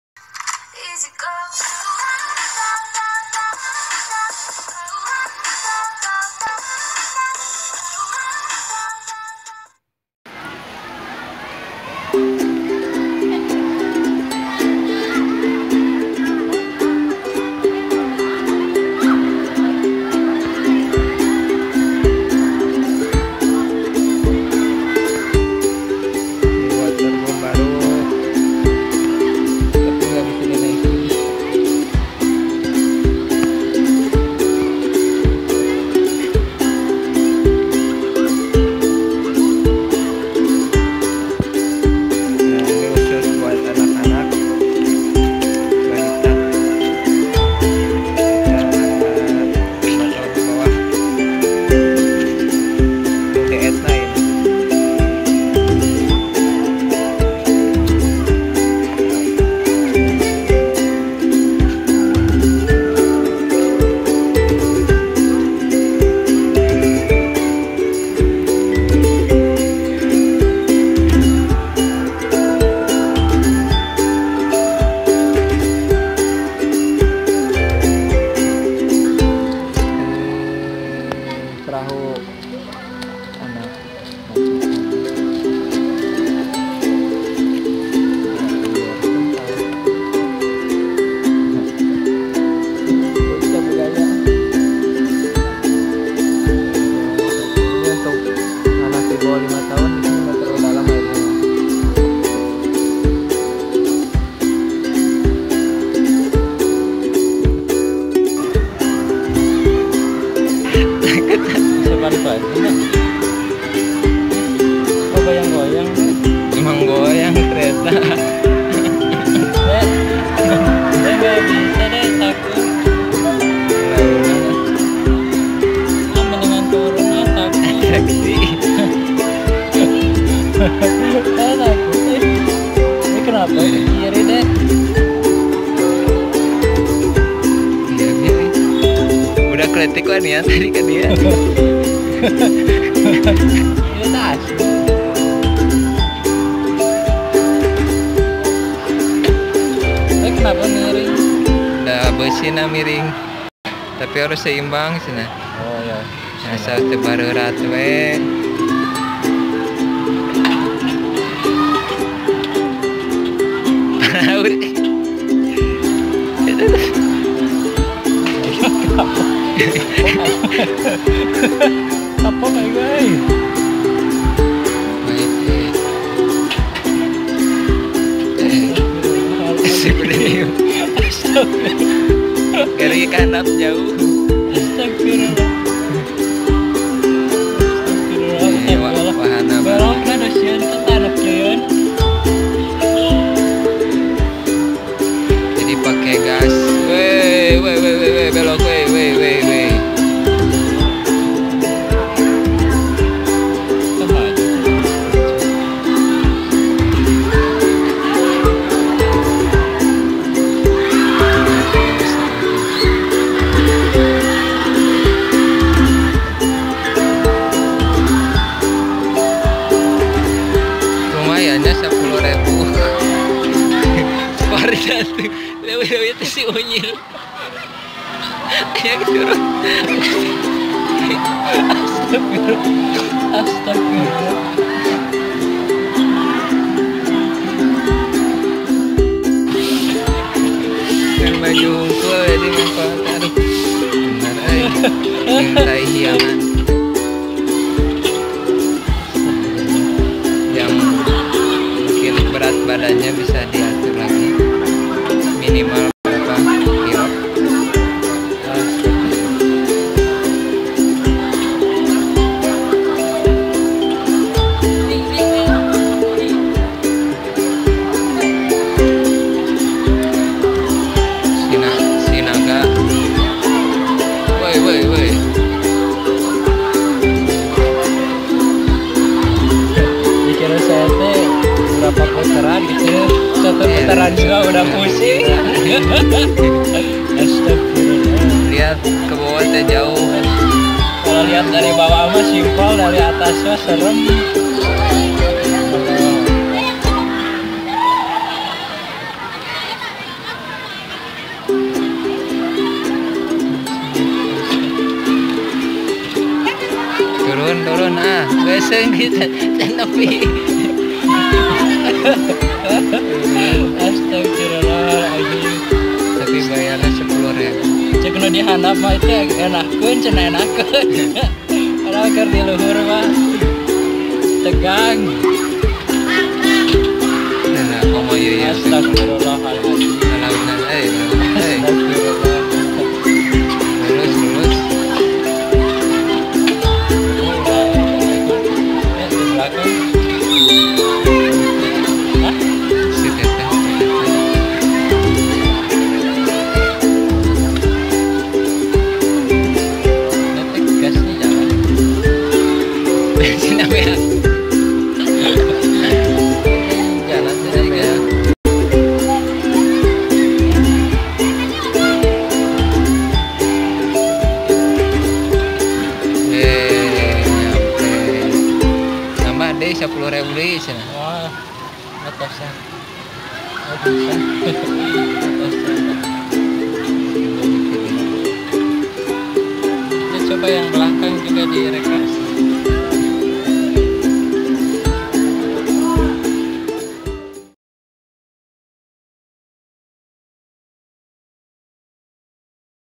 Kolam Renang Karang Setra Waterland dan Sepeda Udaranya
Kolam renang karang setra waterland Bandung lokasi di daerah setiabudi dekat cihampelas walk. Dikenal dengan serodotan naganya, tambah dengan sepeda udara dan waterboom. Kolam renang legendaris di tengah kota.

Tiket masuk 45rb
Untuk naik sepeda udara cukup bayar 10rb perorang.

Alamat : Jl. Sirnagalih No.15
Jl. Sirnagalih No.15, Gegerkalong, Kec. Sukasari, Kota Bandung, Jawa Barat 40162